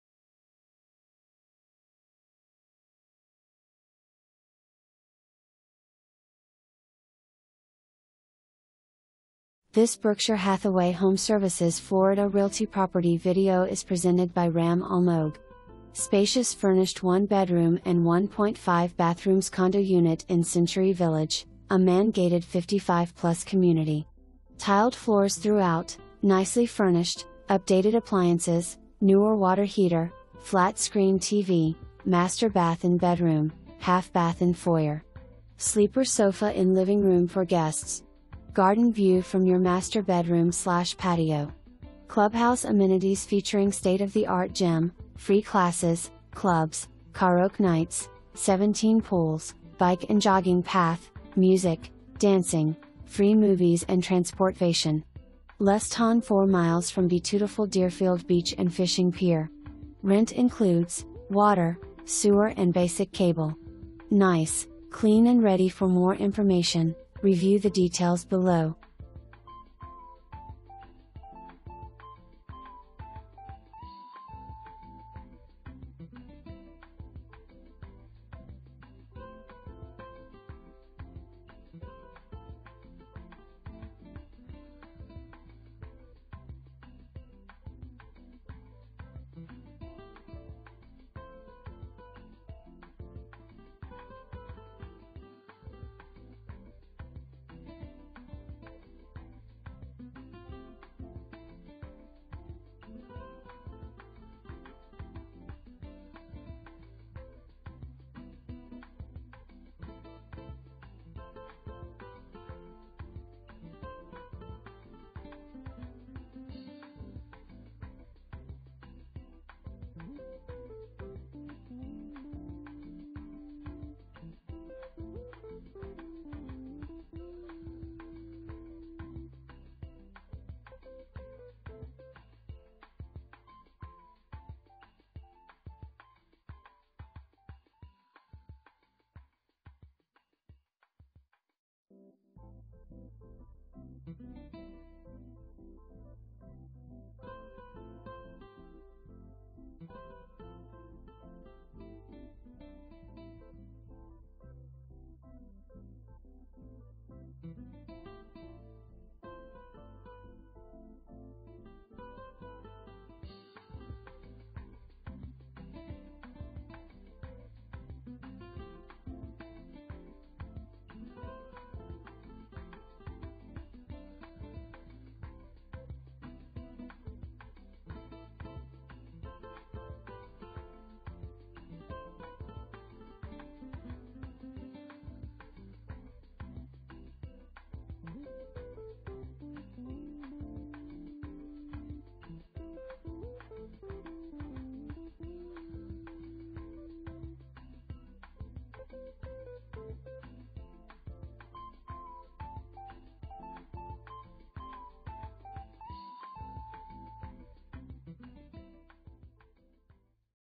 Berkshire Hathaway HomeServices Florida Realty - 32 Ventnor B 32
32 Ventnor B 32
$1,400, 1 bed, 1.50 bath, 700 SF, MLS# RX-10846451

SPACIOUS FURNISHED 1 BEDROOM AND 1.5 BATHROOMS CONDO UNIT IN CENTURY VILLAGE, A MAN-GATED 55+ COMMUNITY. TILED FLOORS THROUGHOUT, NICELY FURNISHED, UPDATED APPLIANCES, NEWER WATER HEATER, FLAT SCREEN TV, MASTER BATH IN BEDROOM, HALF BATH IN FOYER. SLEEPER SOFA IN LIVINGROOM FOR GUESTS. GARDEN VIEW FROM YOUR MASTER BEDROOM/ PATIO. CLUBHOUSE AMMENITIES FEATURING STATE OF THE ART GYM, FREE CLASSES, CLUBS, KAREOKE NIGHTS, 17 POOLS, BIKE AND JOGGING PATH, MUSIC, DANCING, FREE MOVIES AND TRANSPORTVATION. LESS THHAN 4 MILES FROM BEATUTIFUL DEERFIELD BEACH AND FISHING PIER. RENT INCLUDES, WATER, SEWER AND BASIC CABLE. NICE, CLEAN AND READY

Ram Almog
Berkshire Hathaway HomeServices Florida Realty